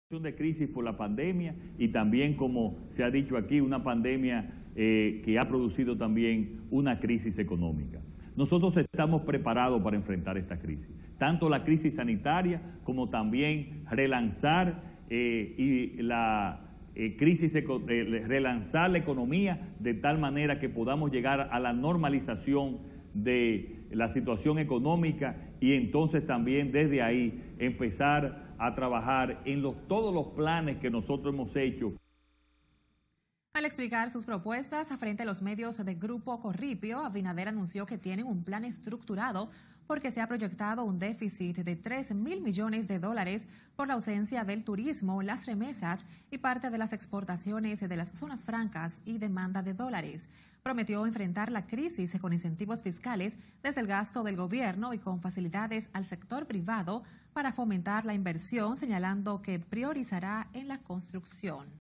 Abinader ahora dice que se necesitan préstamos
El candidato presidencial del PRM, Luis Abinader, dio hoy un vuelco en su posición contra los préstamos internacionales, los cuales han cuestionado al actual Gobierno al que atribuye el endeudamiento del país.